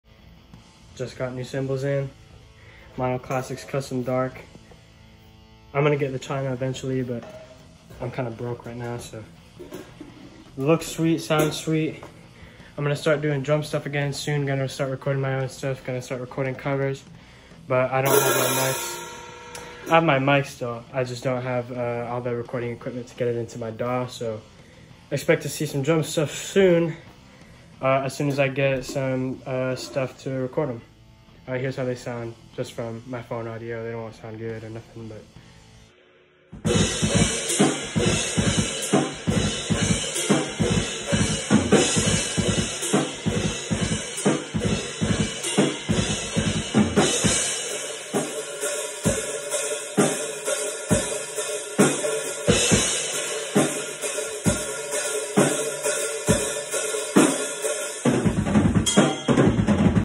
new cymbals! meinl classics custom dark cymbals, expect to see drum stuff in the next month or two!
new cymbals! meinl classics custom dark cymbals, expect to see drum stuff in the next month or two. i just need to get an 8 input interface and stuff like that first and then we’re all set to go for covers, and maybe starting some original projects… 👀